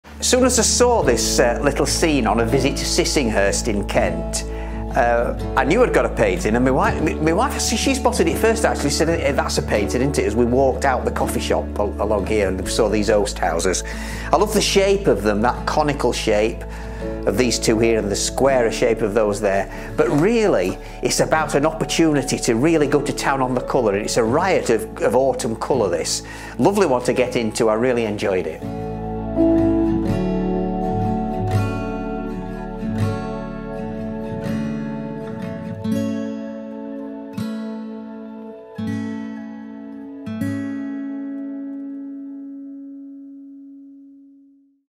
Oast Houses, Sissinghurst - Preview with Geoff Kersey ⎮ Watercolour Landscapes
During a visit to Kent in the autumn of last year, I came across this scene and knew immediately that it would make a really good painting tutorial.

It’s in the grounds of the National Trust property, Sissinghurst Castle, which was the home of the author and garden expert Vita Sackville-West.

I decided that the scene was busy enough without the parasol and seated figures; so I left them out of the composition making the focal point the fascinating shapes of the Oast Houses.

I was also drawn to the warm tones of the brickwork, the interesting shapes of the roofs, and the foliage which has been allowed to encroach on the buildings, providing us with an attractive contrast of hard and soft shapes.

The composition is completed by the large tree on the right; its beautiful autumn colours contrast perfectly with the pure blue of the sky.

A really enjoyable subject.

Happy Painting,
Geoff.